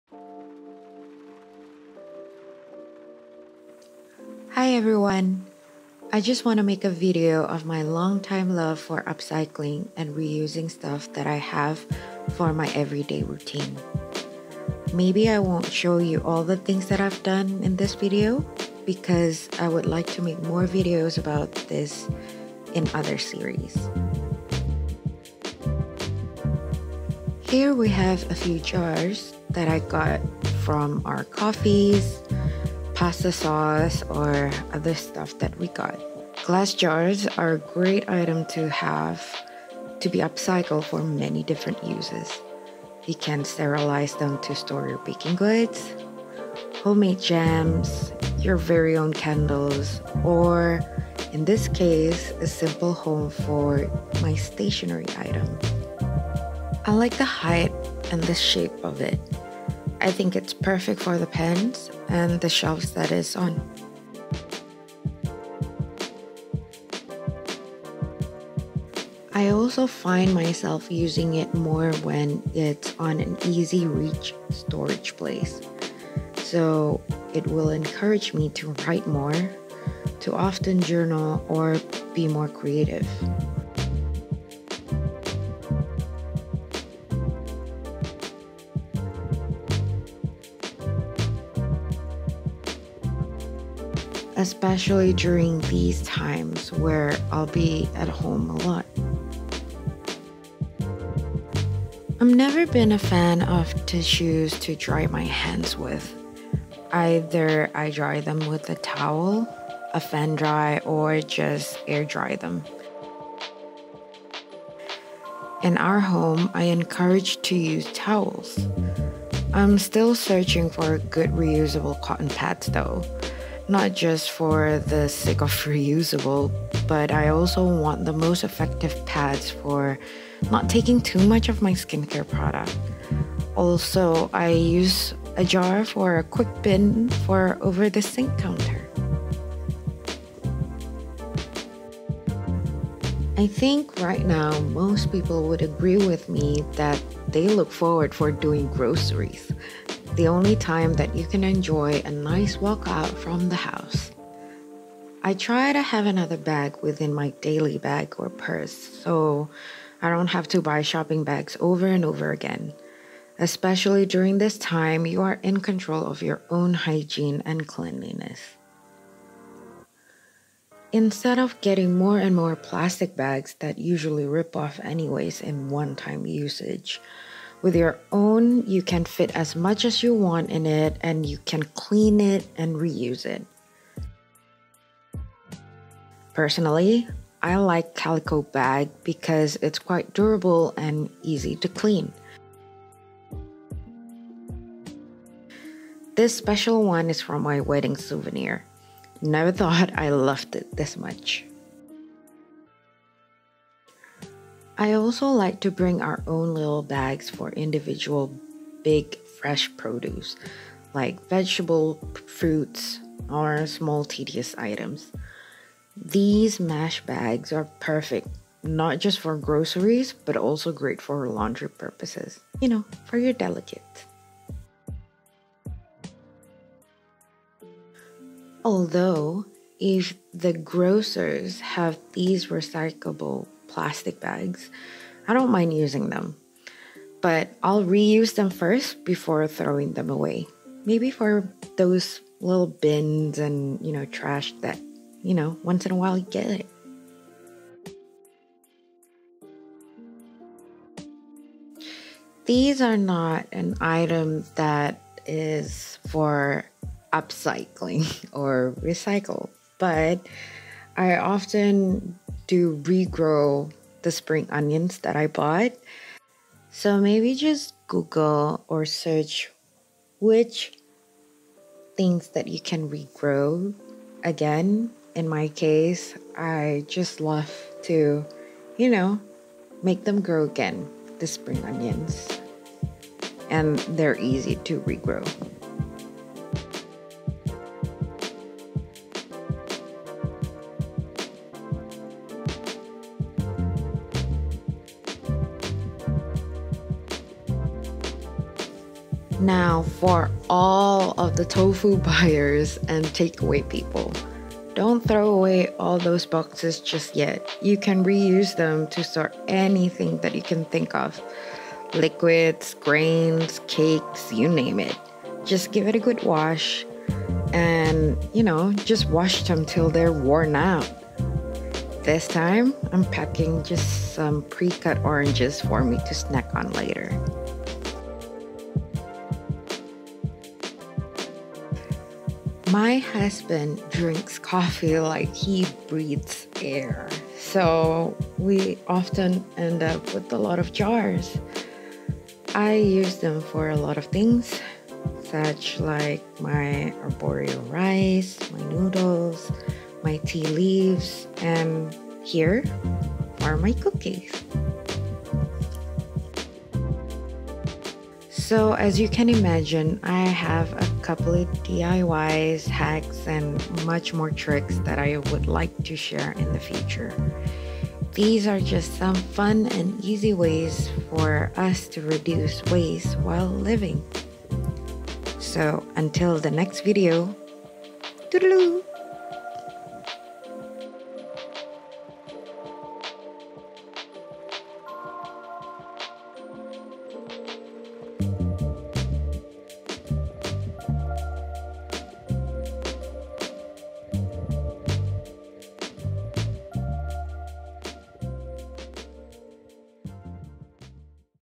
UPCYLING & REUSING | VISUAL JOURNAL
Is anyone like me who wishes that your go-to instinct getting only what we truly need and be content with it? Please emphasize on the word 'wishes', because I am a sucker on certain aesthetics, taste and looks. Although, I have been trying to be more aware of the new purchases that I make on a regular basis.

The whole system buying in bulks can really be so tempting. Who doesn't love a good Costco bargain? who doesn't like that buy 1 get 1 thing free? When you do it smartly, yes you can save quite some coins; but with what cost... ?

I hope this can inspire to start your own journey to more recycling, upcycling or more mindful buying.  Let yourself to enjoy the process as well. All for a better place for us to live and healthier space.

EDITED BY
Amanda Harsono